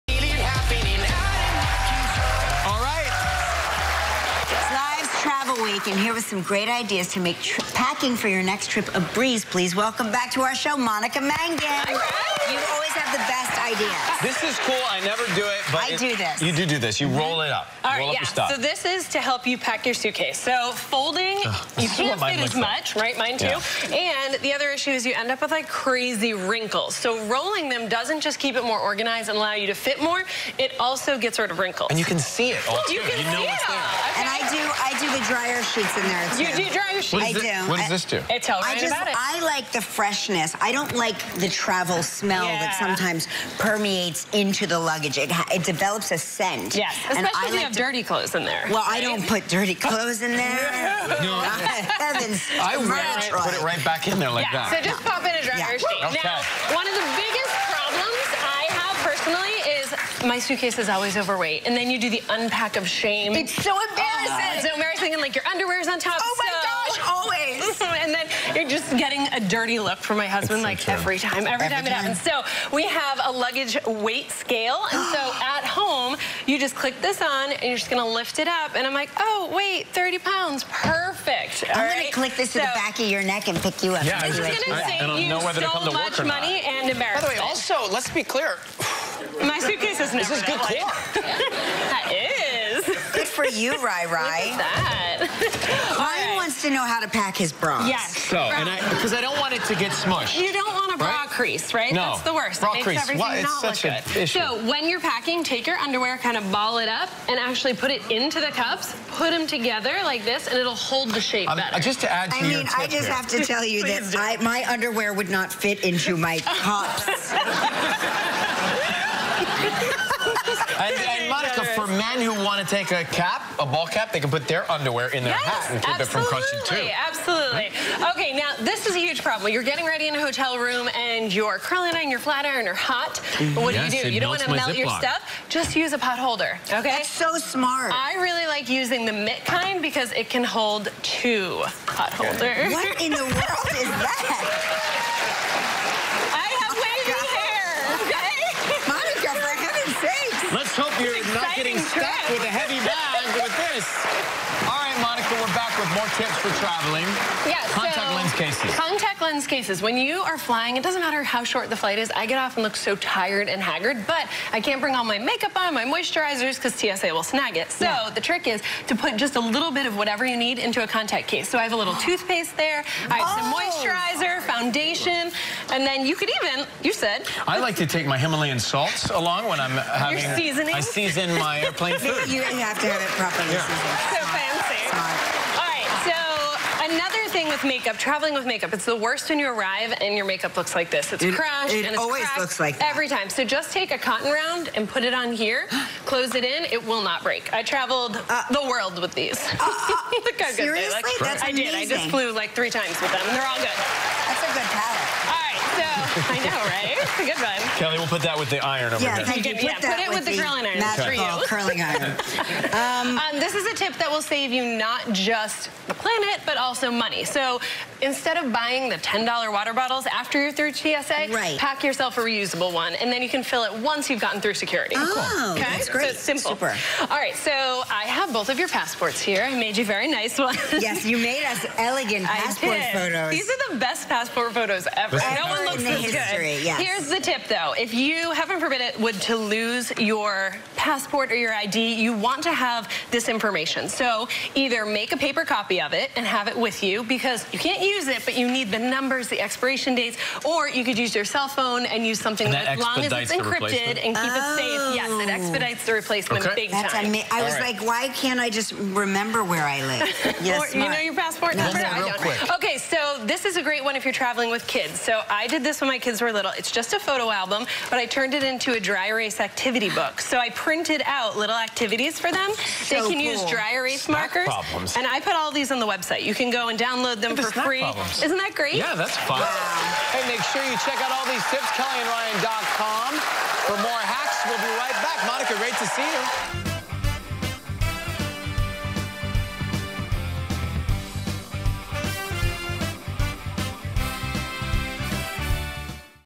Travel Week: Travel Hacks with Monica Mangin
Monica Mangin shares several handy tricks to make travel easier.